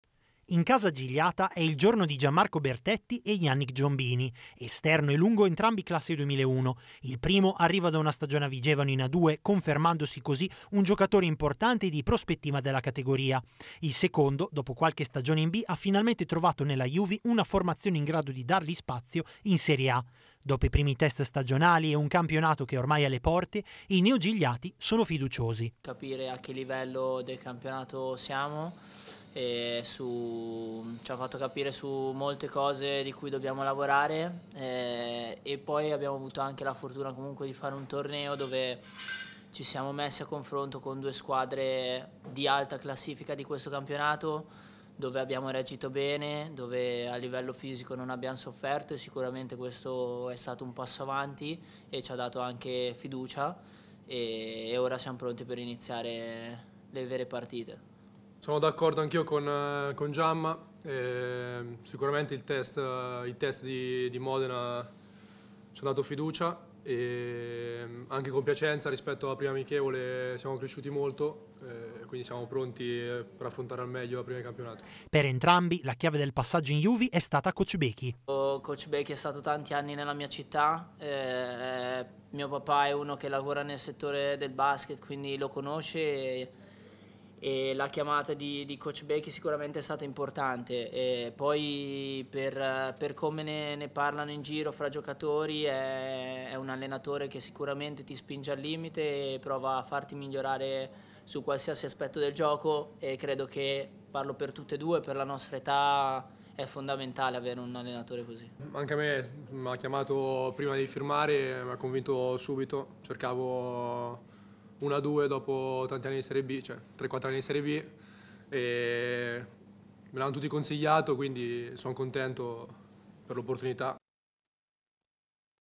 Juvi, ecco Bertetti e Giombini
un playmaker e un centro per il futuro: la Juvi Cremona ha presentato alla stampa Gianmarco Bertetti e Yannick Giombini